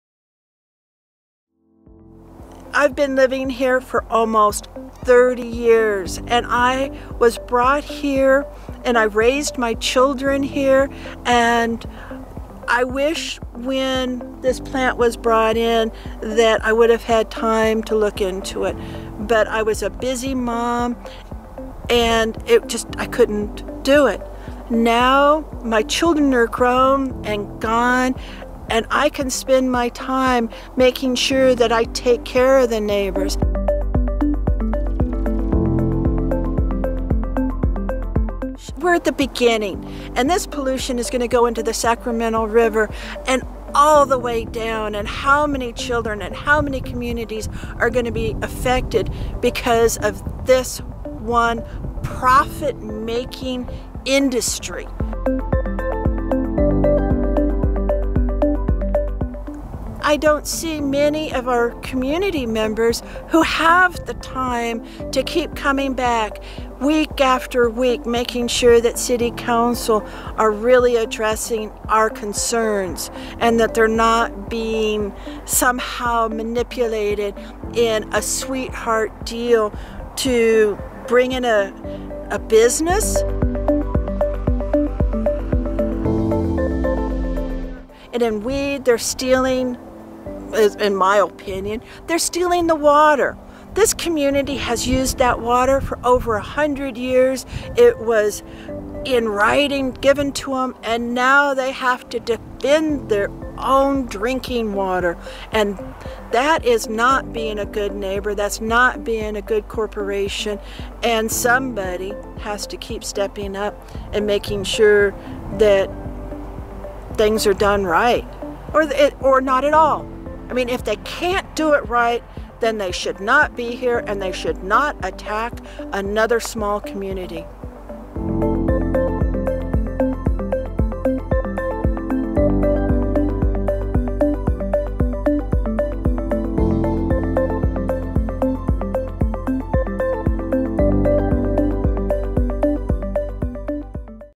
W.A.T.E.R. Treasurer Roslyn McCoy: Effects of Crystal Geyser on the environment and the community
Roslyn McCoy, W.A.T.E.R. Board Member, shares her thoughts on the effects of the Crystal Geyser plant on the Sacramento river and the surrounding Community.  Please support and donate to  W.A.T.E.R. this Giving Tuesday Nov.27, 2018  to support their efforts on protecting Mt.Shasta Water from Corporate control.